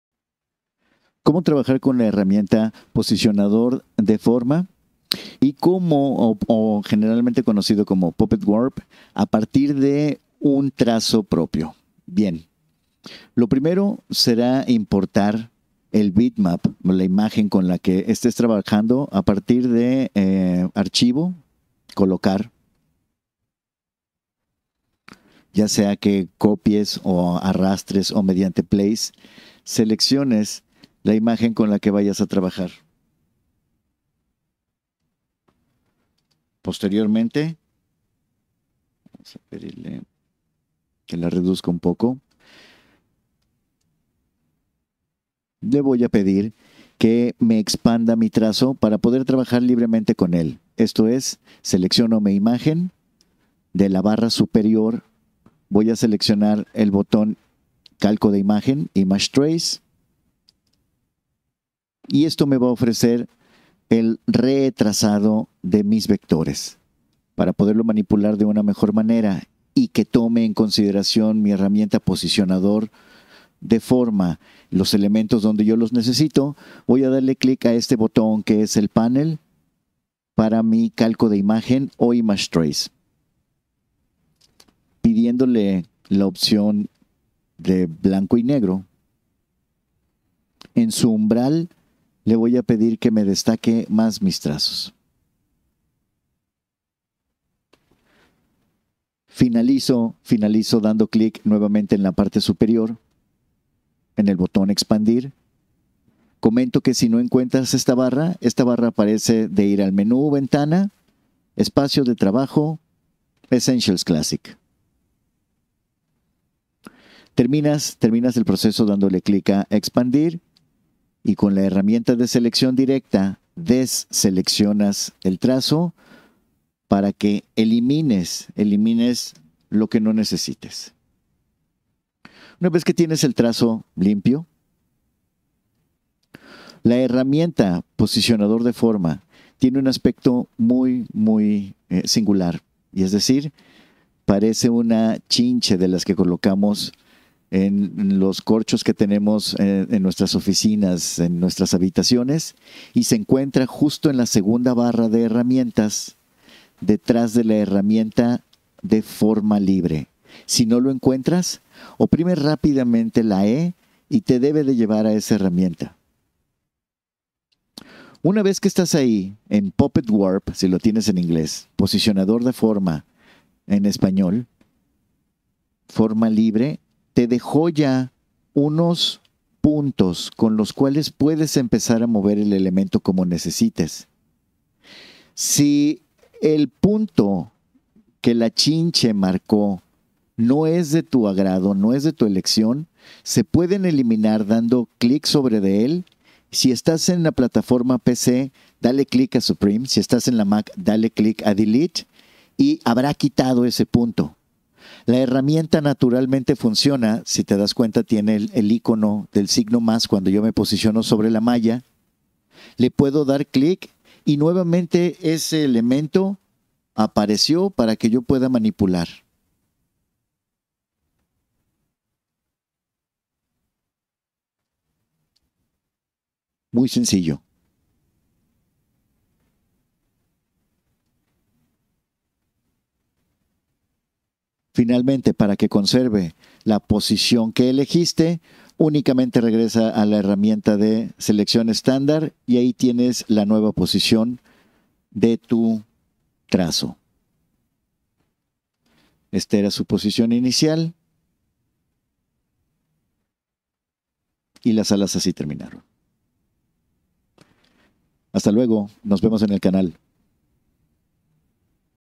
PUPPET WARP (POSICIONADOR DE FORMA LIBRE)
Como si fuera una marioneta puedes cambiar la posición de tus elementos como necesites, ideal para bocetos de personajes y cambio de posiciones